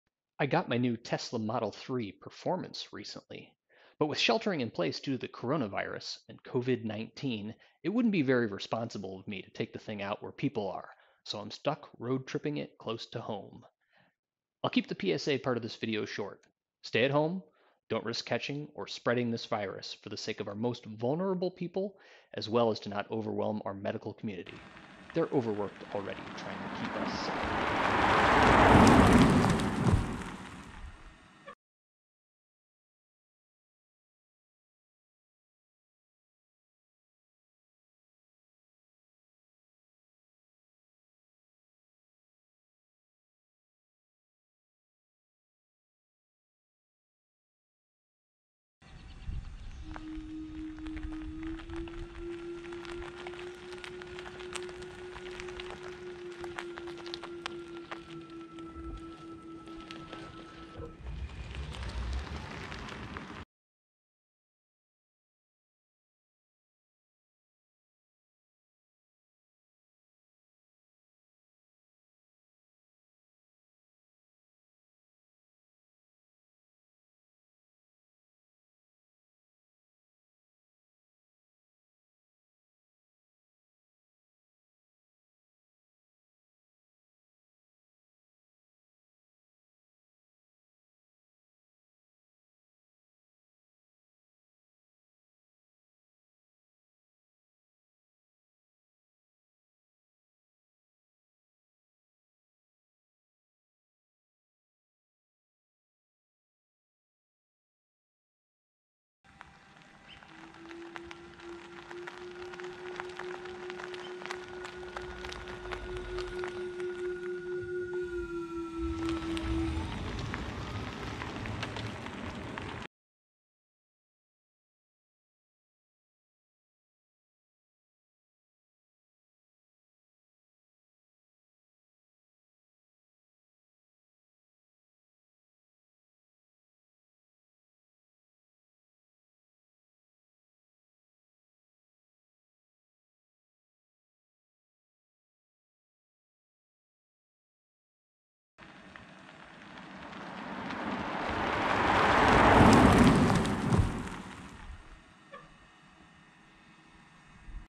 Road Tripping with my Tesla Model 3 During the COVID-19 Epidemic
Having fun at home and trying to stay sane while sheltering in place. Stay safe and healthy my friends, flatten the curve!

Equipment used for filming:
GoPro 7 Black
DJI Mavic Mini